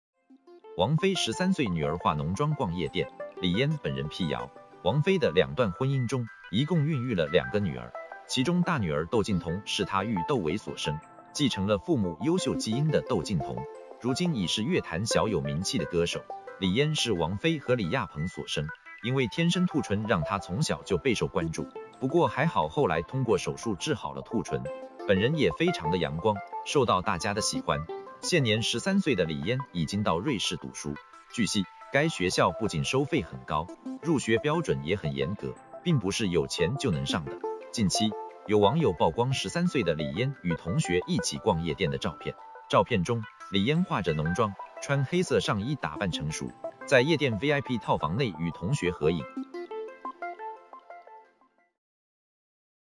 王菲13岁女儿化浓妆逛夜店？李嫣本人辟谣
➤Thanks收看！